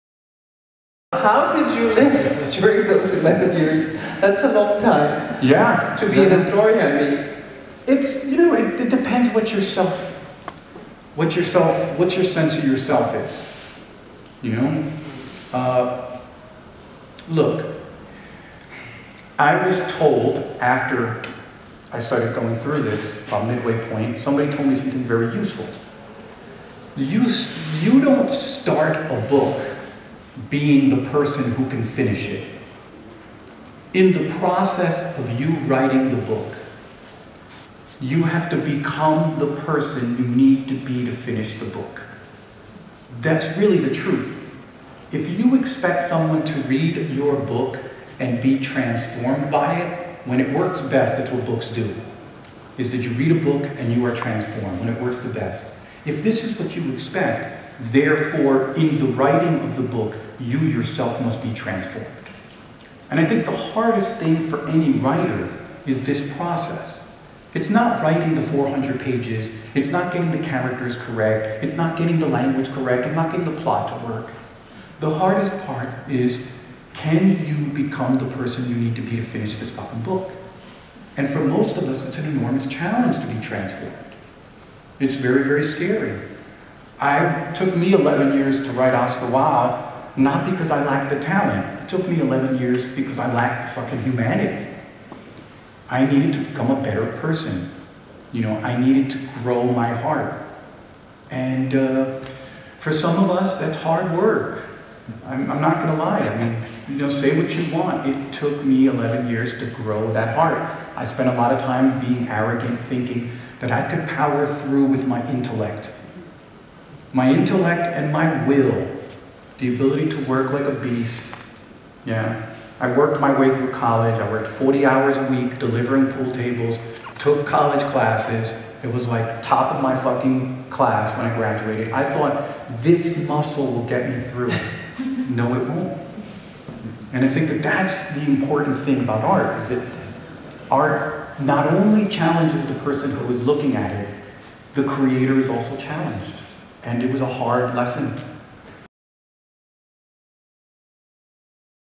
Oscar Wao's tilblivelse
Det tog Junot Diaz 11 år at skrive bogen om Oscar Wao. Hør ham fortælle, hvordan det er at leve med en roman i så mange år, og hvorfor han var så langt tid om at afslutte den.